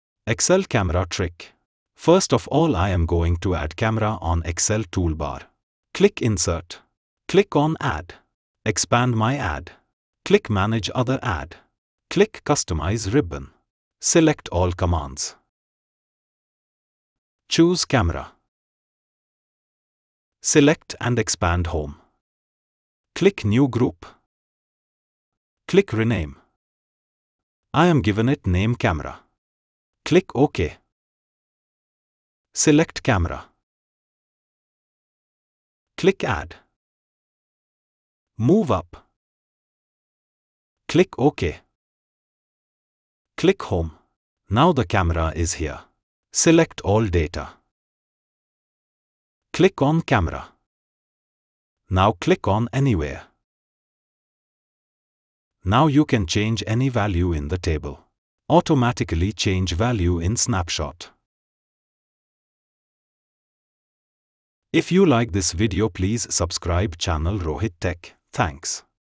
Camera Trick To work Smart in Excel. !! M365, Office365, MSO
The Camera tool in Excel is a hidden but powerful feature that lets you take a live snapshot of a selected range of cells and place it elsewhere in the workbook. The snapshot updates automatically if the original data changes.

📌 Use Cases
Create dashboards or reports.
Display data summaries in another sheet.
Track changes dynamically.